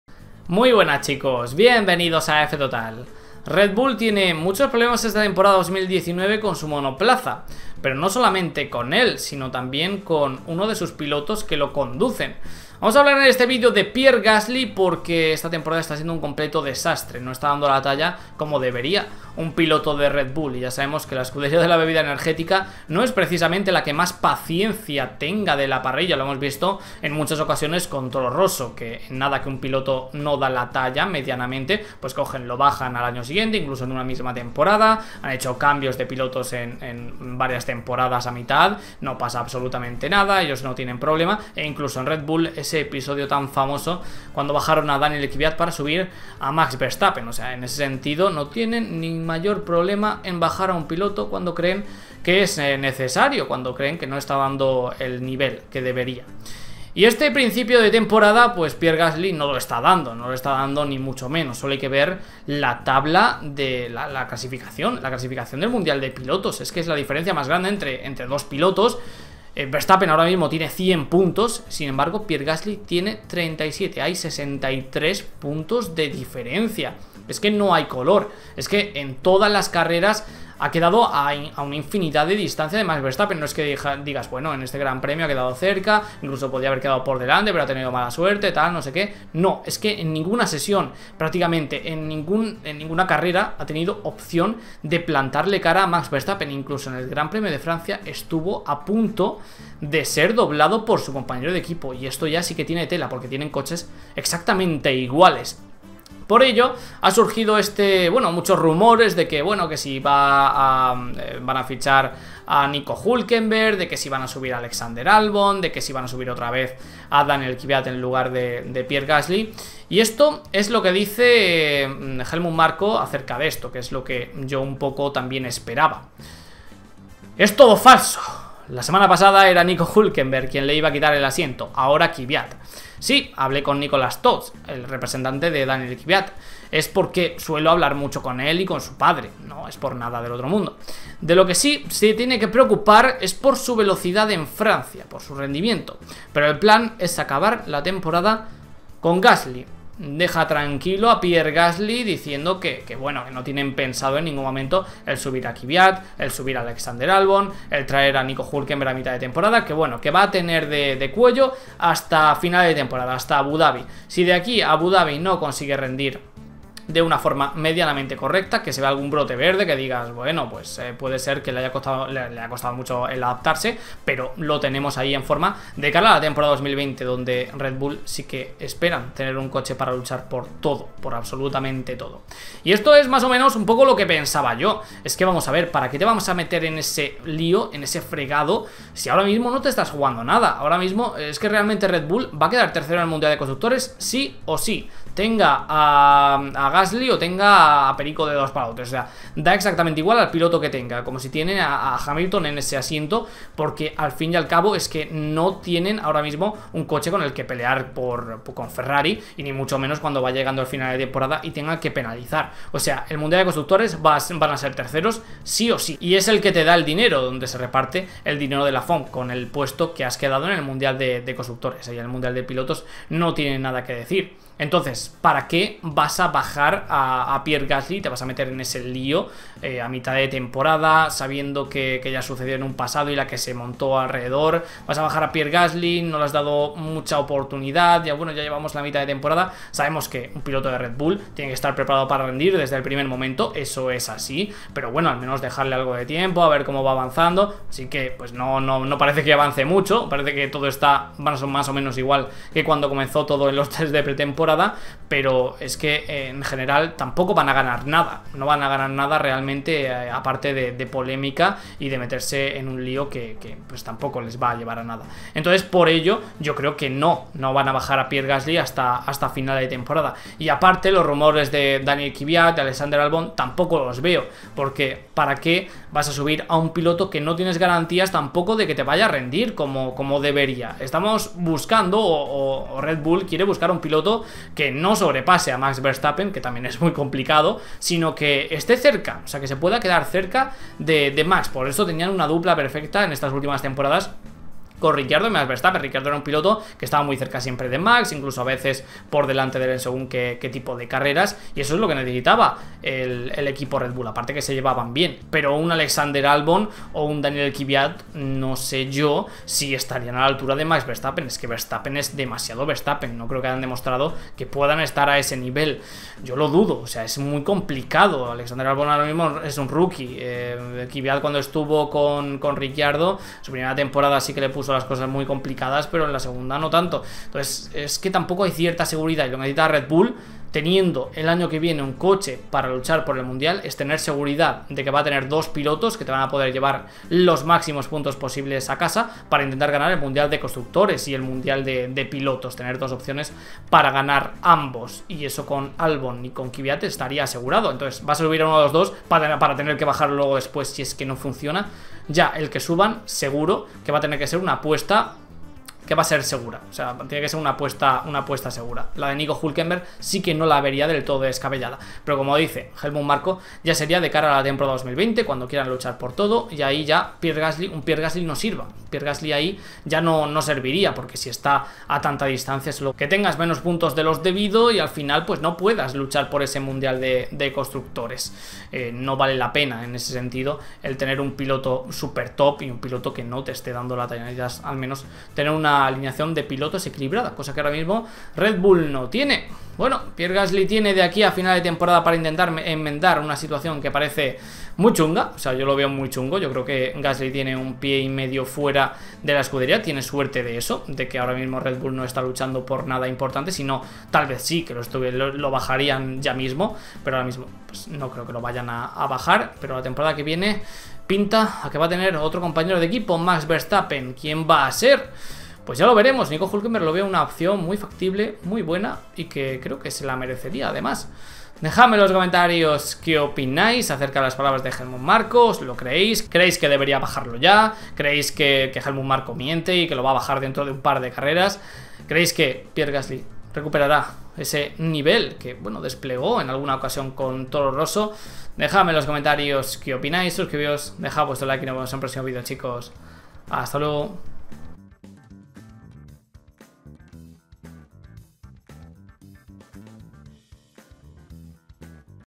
GASLY CON EL AGUA AL CUELLO... ¡RED BULL SE EMPIEZA A CANSAR! ¿¿KVYAT?? ¿¿ALBON?? ¿¿HULKENBERG??
RESUMEN CARRERA GP FRANCIA F1 2019 - HAMILTON ARRASA, BOTTAS SUFRE CON LECLERC Y SAINZ BRILLA F1 2019 FORMULA 1